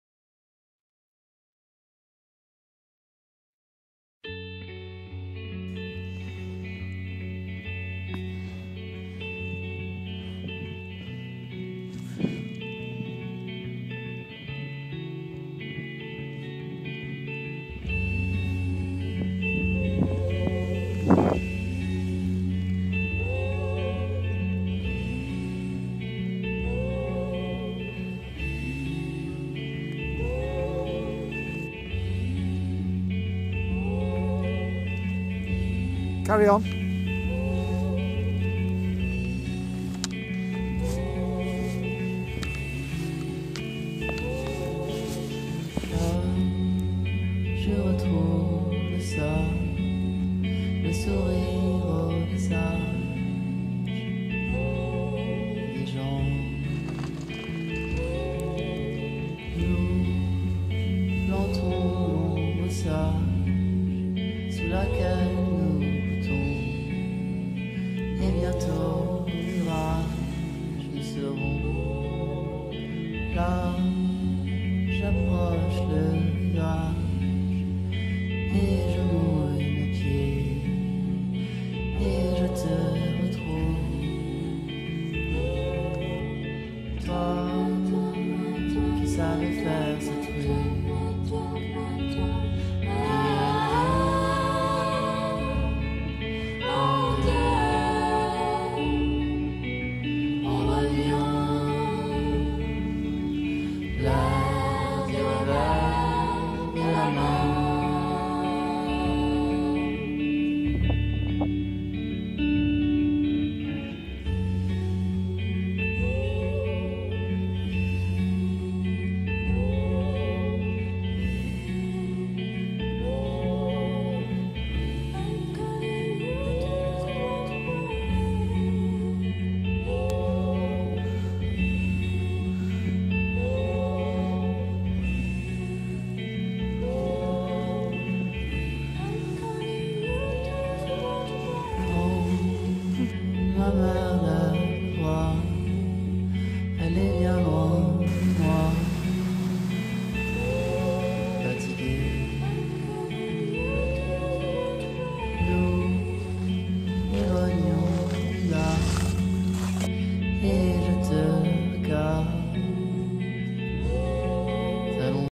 Family Ski Company Snowy Powder day Ardent Portes du Soleil
Family Skiing Holidays for all the family to enjoy - especially with the fantastic powder skiing conditions of December - the first week of the season.